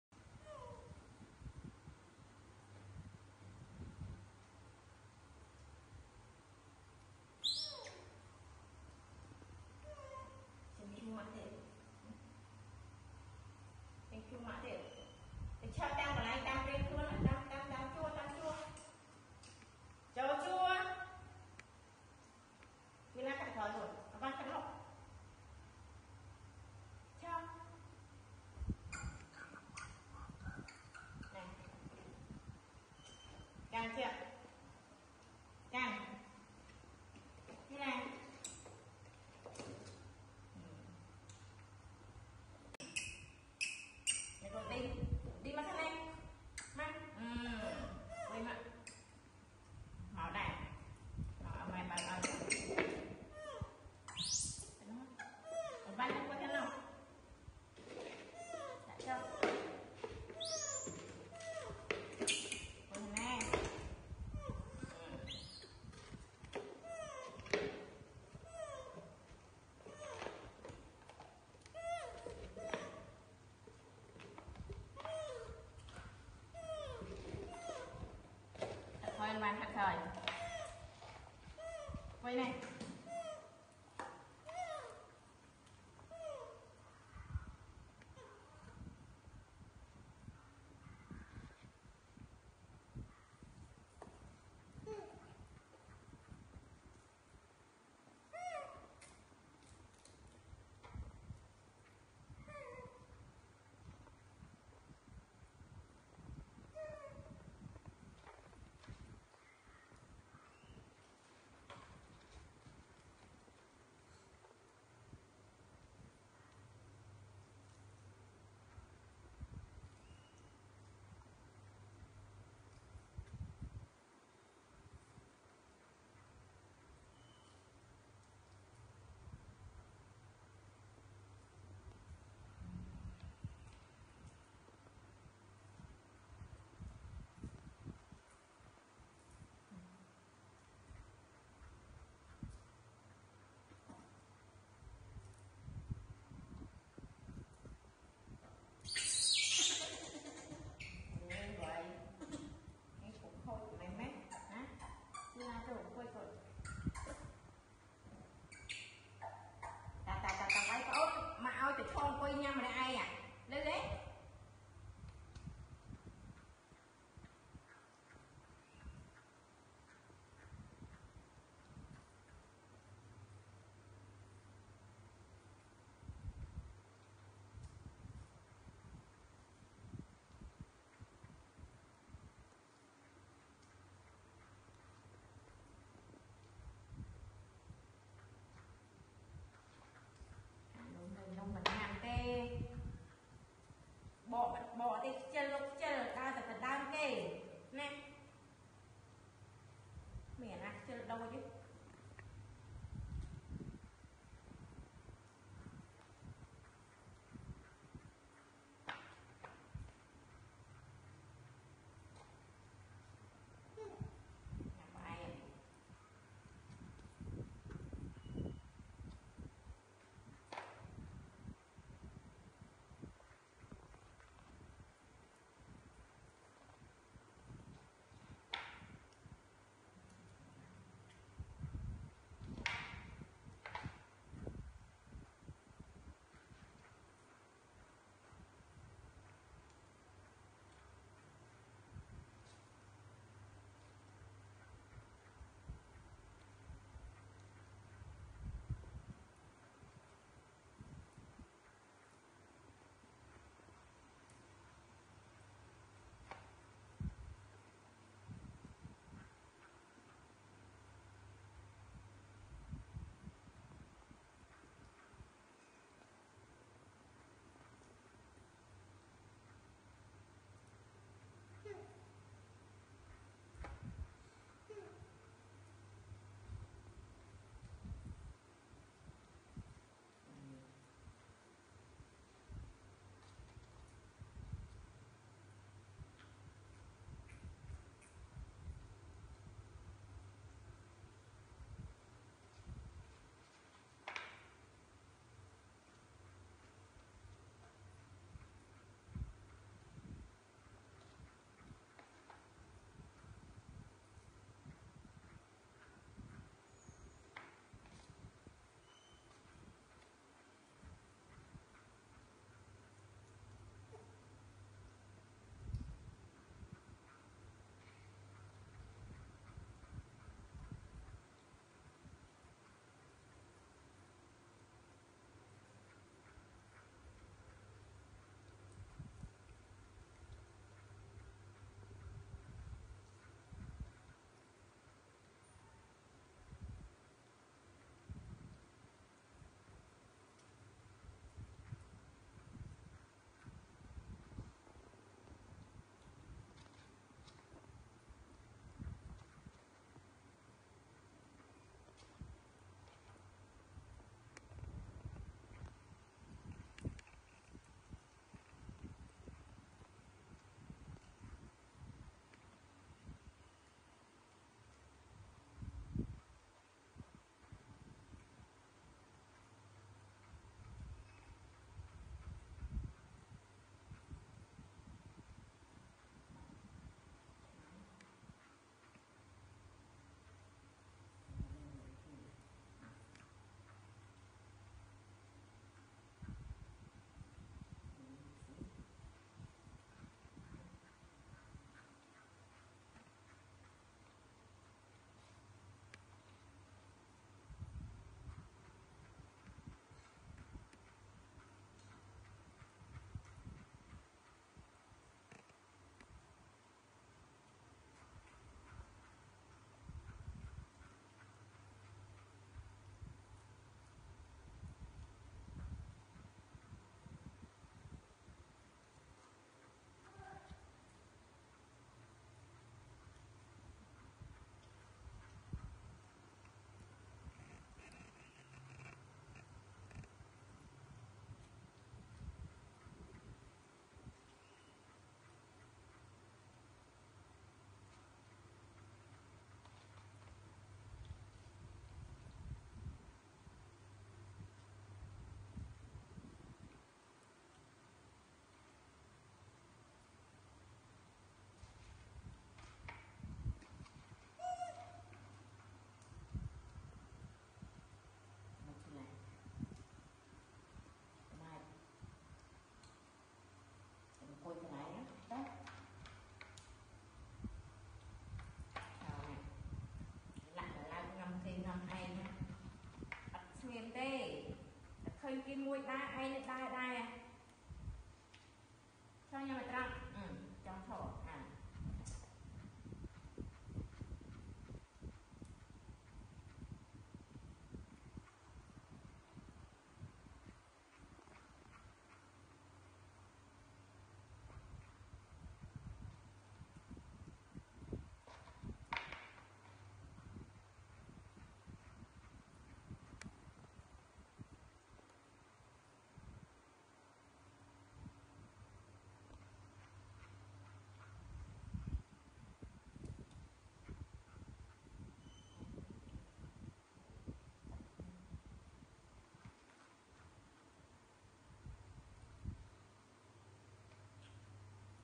4 Siblings Look So Surprise & Curious While Mom Bring Them Mysterious For Dinner, WHAT IS IT?
4 Siblings Look So Surprise & Curious While Mom Bring Them Mysterious For Dinner, WHAT IS IT?